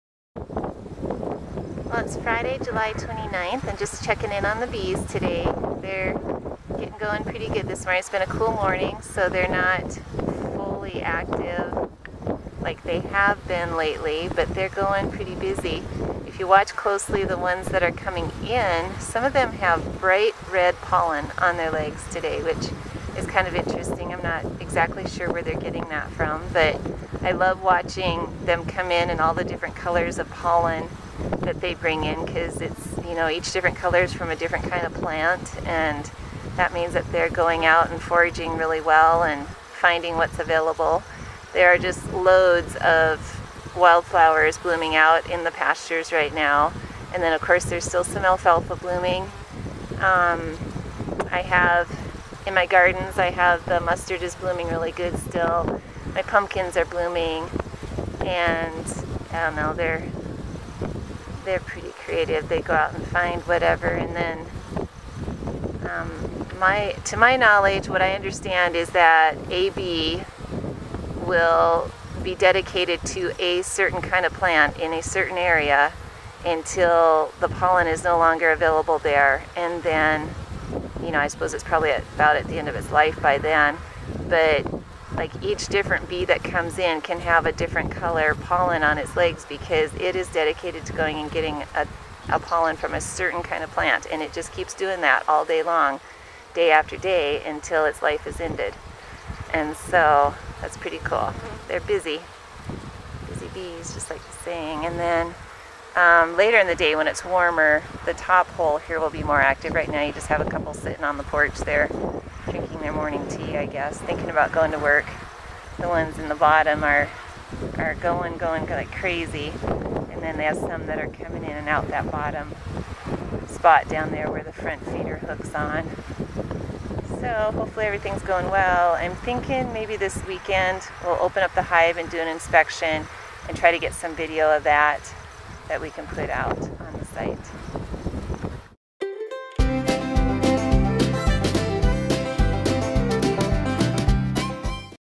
The Bees Front Porch
This short movie shows honey bee activity as they come and go from their hive.